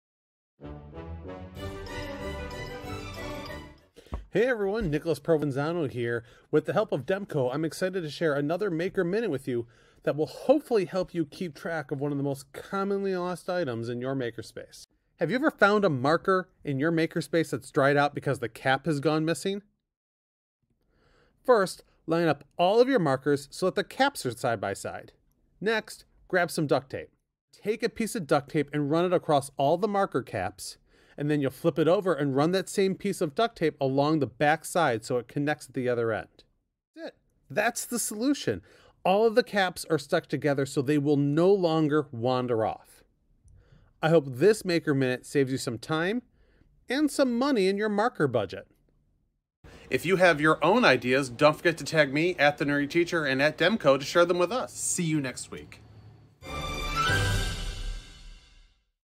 Demco MakerMinute | Missing Marker Caps
Are you replacing dried-out markers more often than you should? This tip is for you!

Do you have a MakerEd idea of your own? Tag @thenerdyteacher and DemcoMaker — we are all better when we learn together!